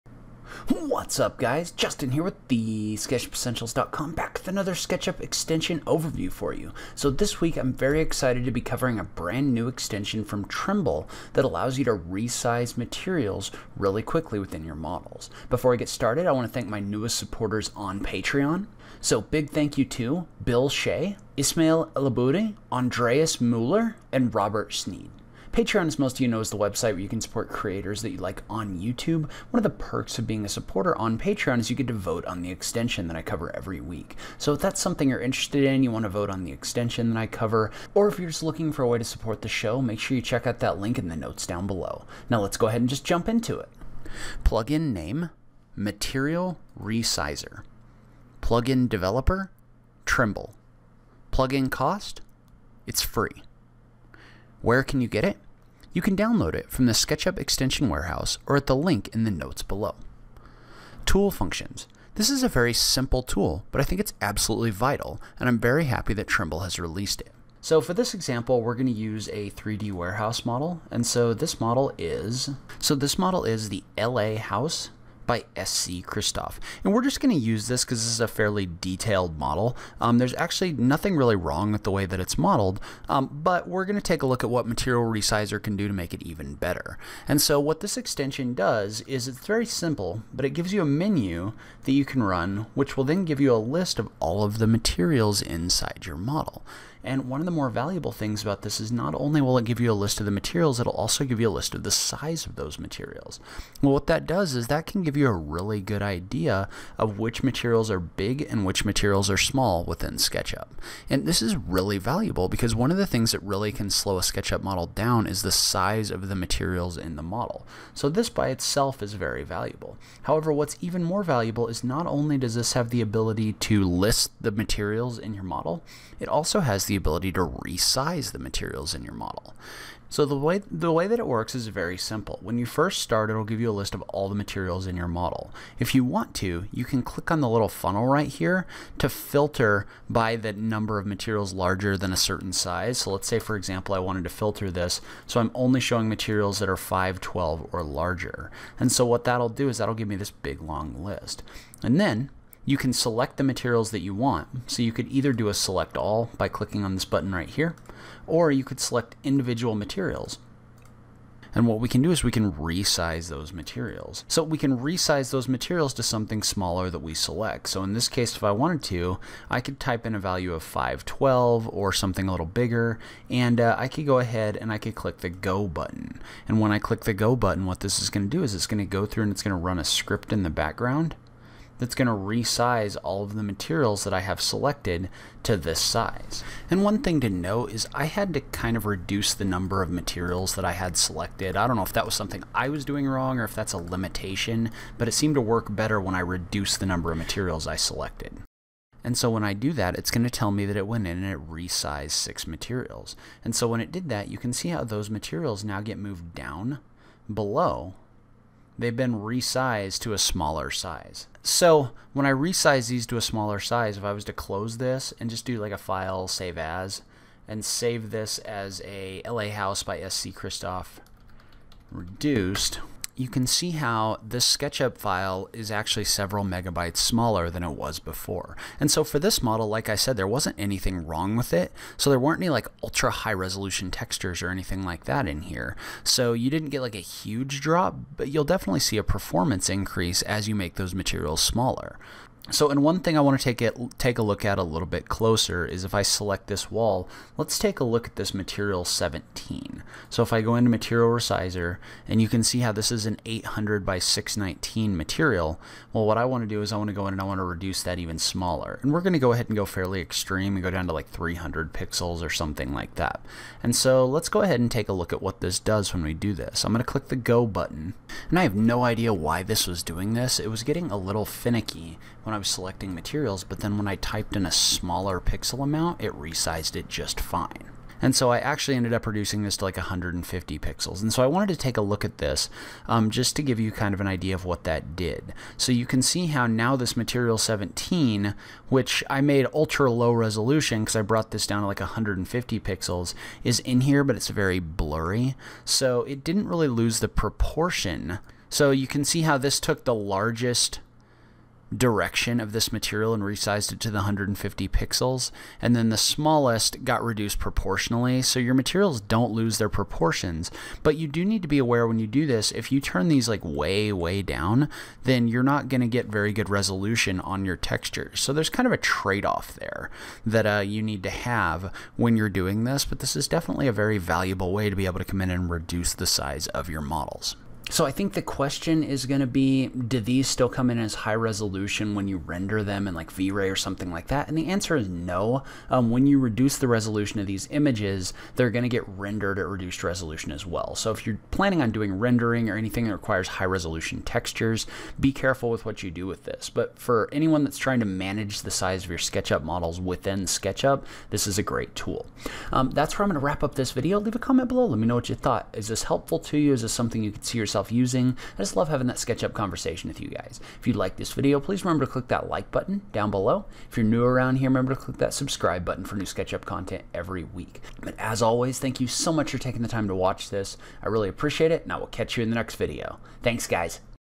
Material Resizer for SketchUp
Material resizer is a brand new extension from Trimble that allows you to quickly view and resize all of the different materials in your SketchUp models. This is especially effective for reducing the size of your models, and for managing the materials from models you've downloaded from the 3D Warehouse!

This is a very simple tool, but I think that it’s absolutely vital and I’m very happy that Trimble has released it.
What it does is that you run the extension, then it gives you a list of all the materials contained within your Sketchup model, as well as information about the size of those materials.
This is very valuable, as one of the things that really slows a SketchUp model down is the size of the materials in the model.
However, what makes this even more valuable is that not only can you view the size of the materials contained in your model, you can also RESIZE those same materials.
There are a couple places this would be valuable. First, and probably most important to day to day users, would be the ability to reduce material sizes within models from the 3D warehouse. The reason for this is that you currently don’t really have any control over the way that objects in the 3D Warehouse are modeled, so sometimes you’ll find a model in the Warehouse you want to use, and it will be really big and really slow.
Usually this is for one of two reasons – either the geometry is heavy, or the images making up the textures in the model are heavy.
Now it’s really easy to download a model, find all the materials over a certain size, and resize them.